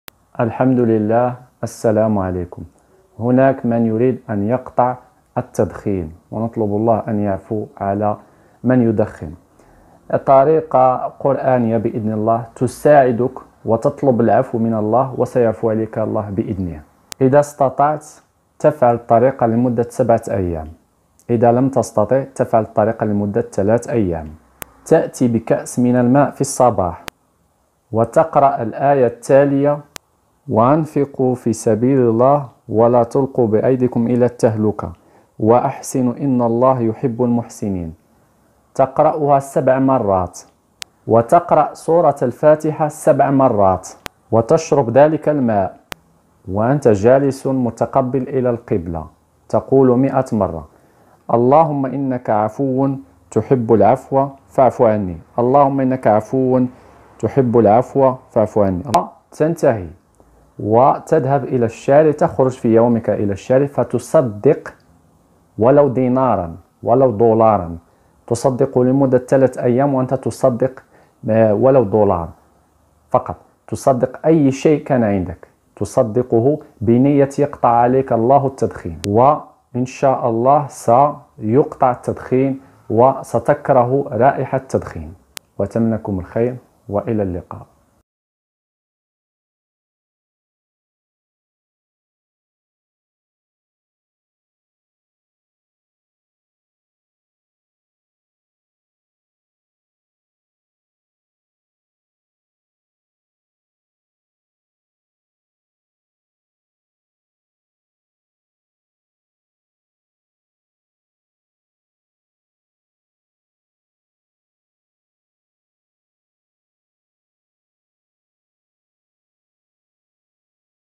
بهذه الآية قصيرة إقرأها بعد صبح وكأنك لم تدخن من قبل ولا تفكر فيه أبدا
أيه المعجزات قصيرة إقرأها بعد صبح وكأنك لم تدخن من قبل ولا تفكر فيه أبدا